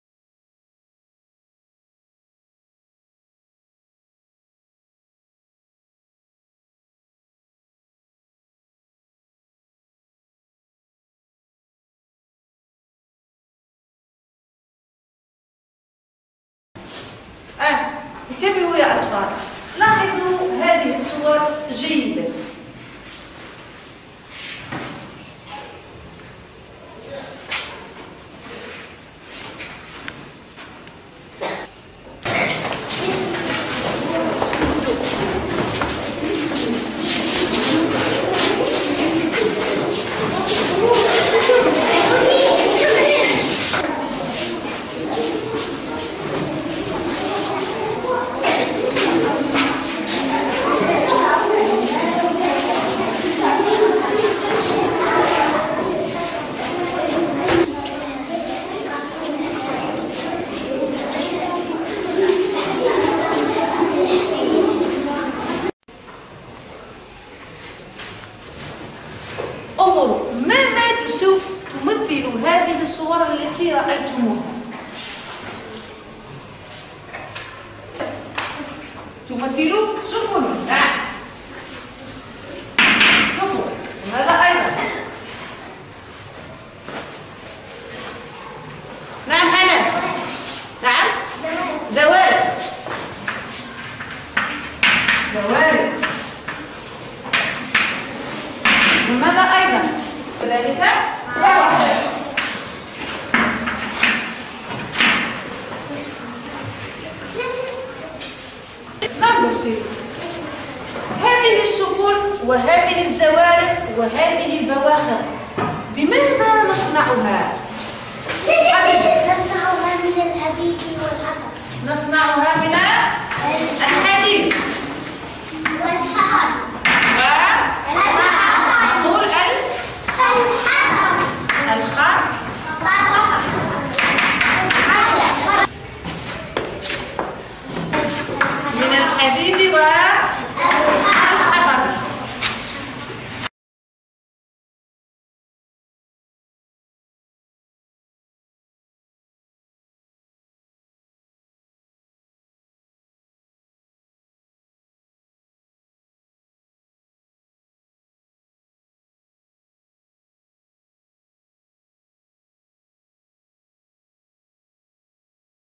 l'école d'antan 2°partie x264
Une séance de cours durant l'année 2004,lorsque l'école était une école et lorsque les élèves étaient des élèves.